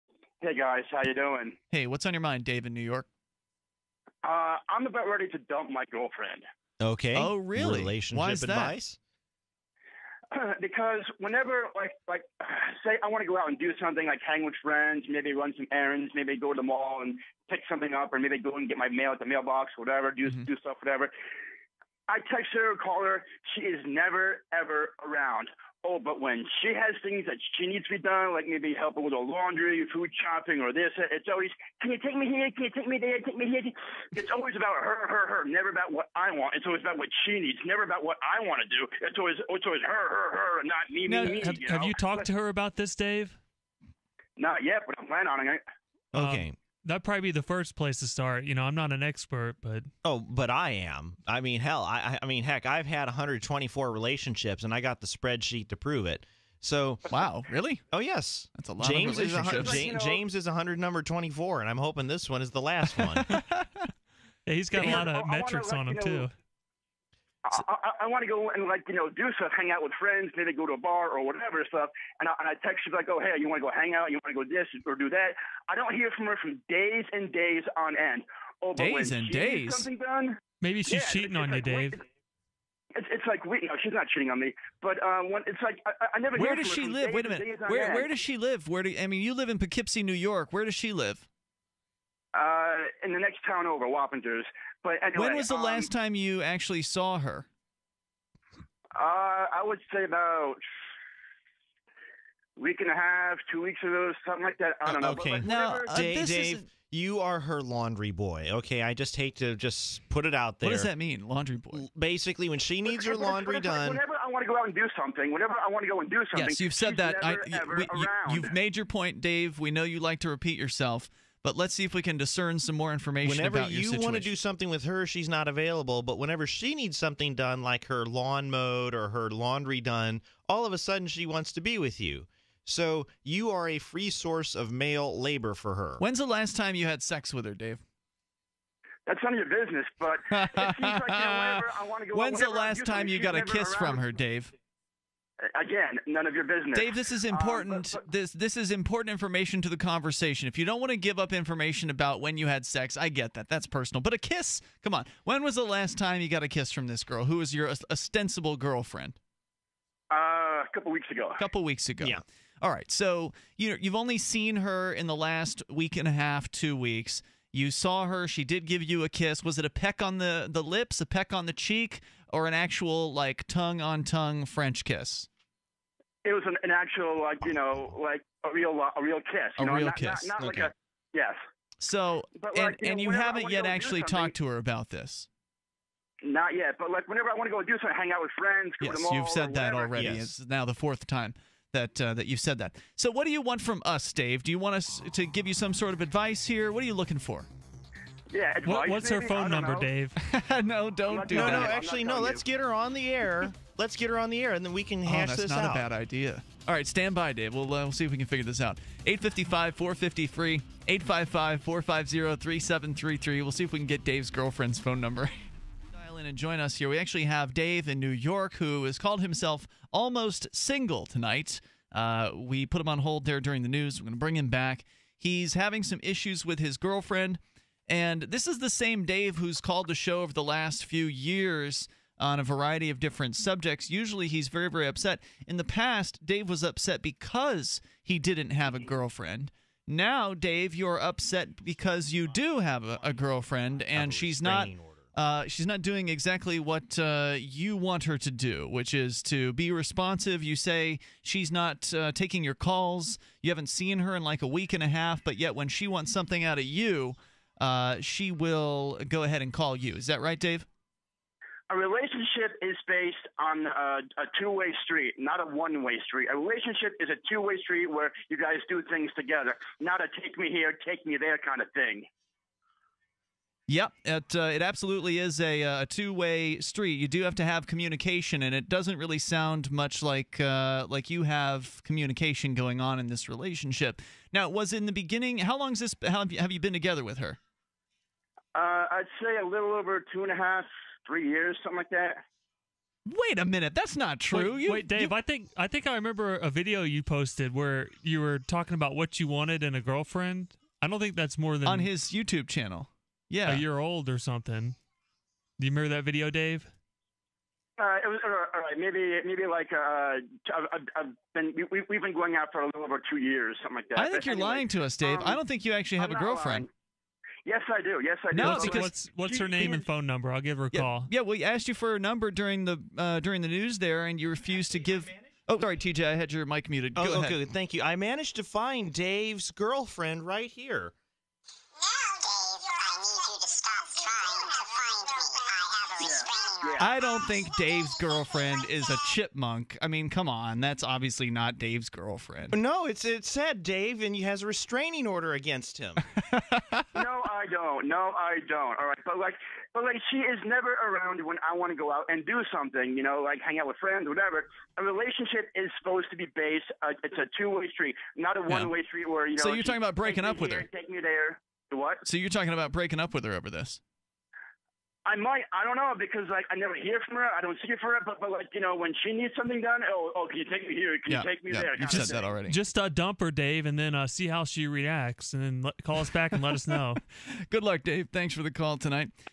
Dave wants to dump his girlfriend
Dave from New York calls in to announce he is going to dump his girlfriend. Ian and TJ try to arrange an on-air call with both of them, and question if Dave is even being honest about having a girlfriend.